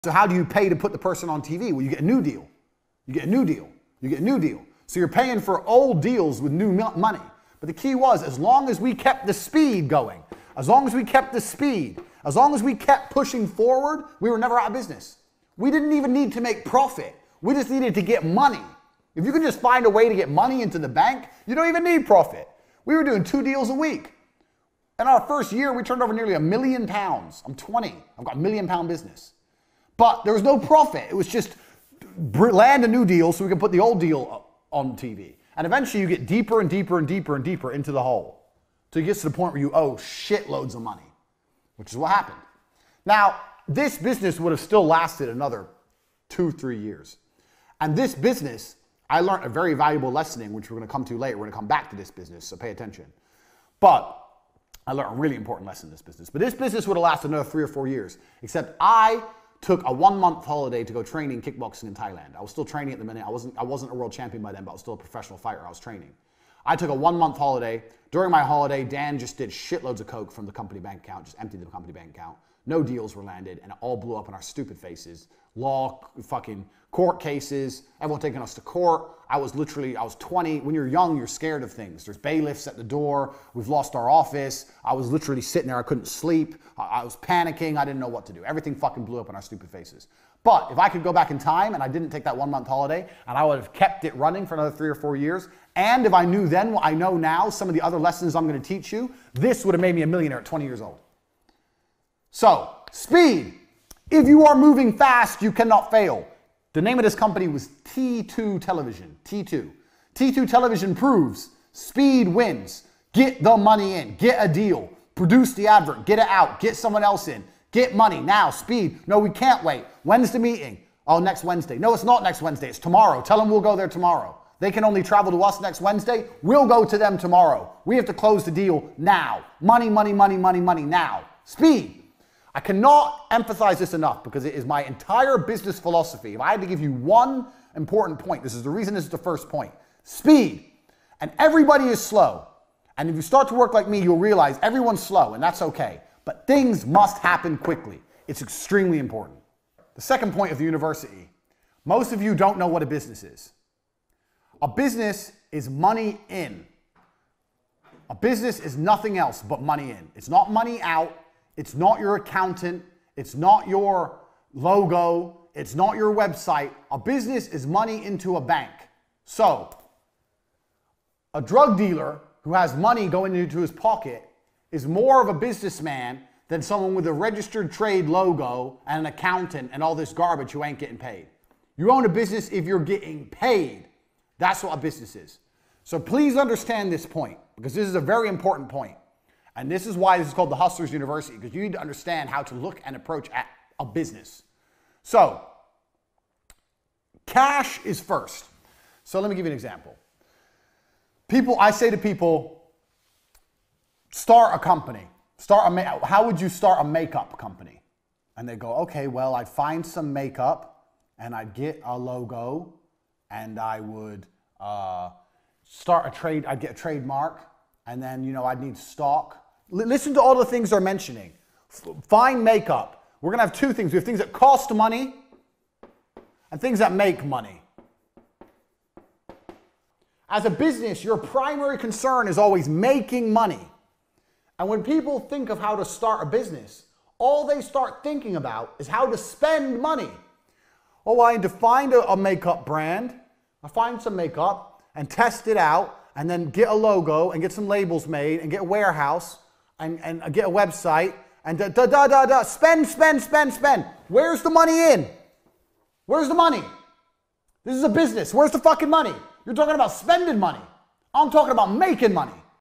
Discover the Power of Speed, Control, and Money in Business with Andrew Tate
In this video, Andrew Tate shares his insights on how to unlock the power of speed, control, and money in business. He reveals his top tips and strategies for achieving success quickly and maintaining control over your business, even as it grows. Whether you're just starting out or looking to take your business to the next level, this video is a must-watch for anyone looking for practical advice and motivation to succeed.